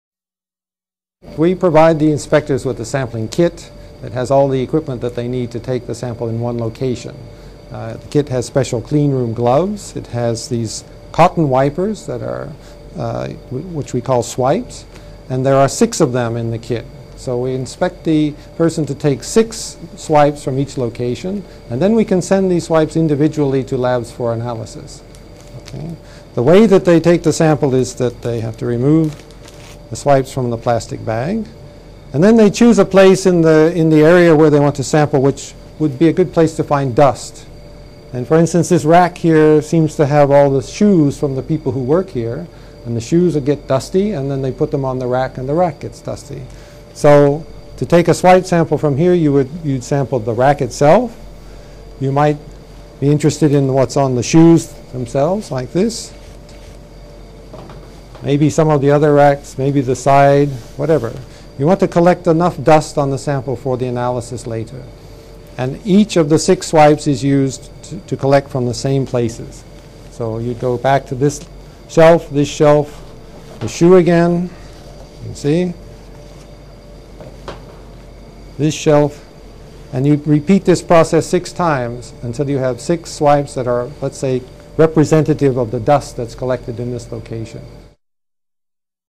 How IAEA Inspectors Swipe to Detect Nuclear Materials
David Donohue, head of the IAEA's Environmental Sample Laboratory, demonstrates how agency inspectors use swipes to detect nuclear materials during safeguards inspections.